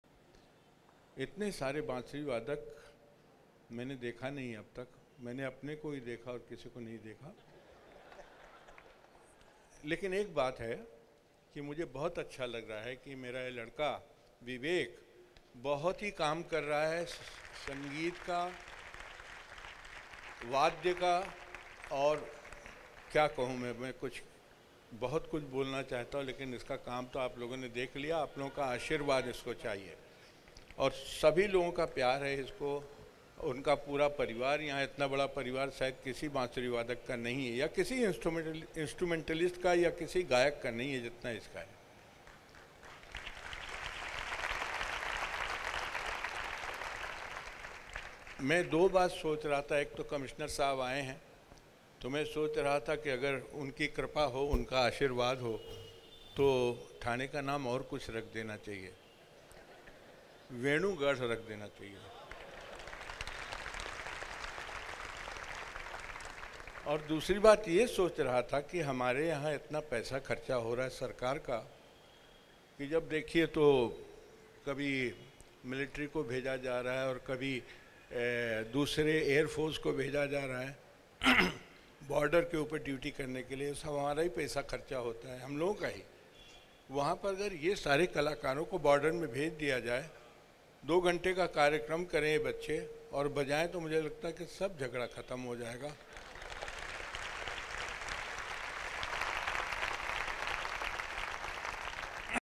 Pt.Hariprasad Chaurasia named Thane as "Venu Garh" | Vivek sonar
Guruji Pt. Hariprasad ji Chaurasia named Thane as "Venu Gadh"